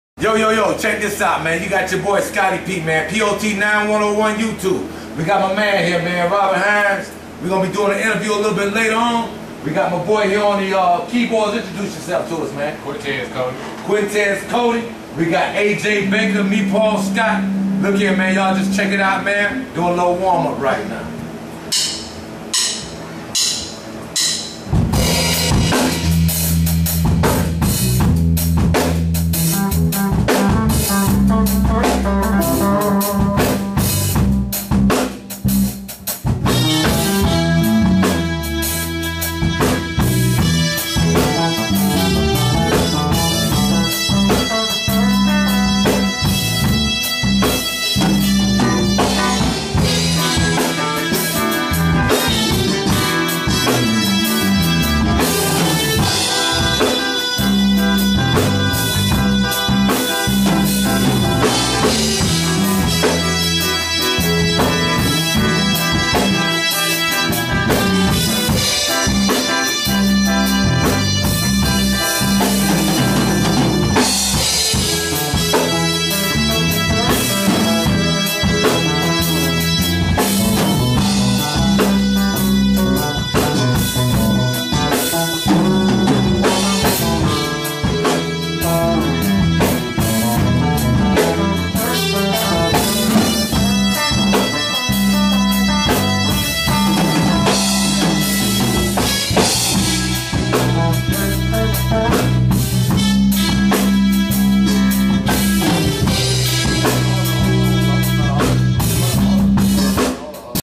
jamming with robert hines-warm up
a two day cover  music project featuring Ashley Stevens clara jefferies aj baker paul scott
cortez cody featuring robert hines camera tech jaqualya scott osborne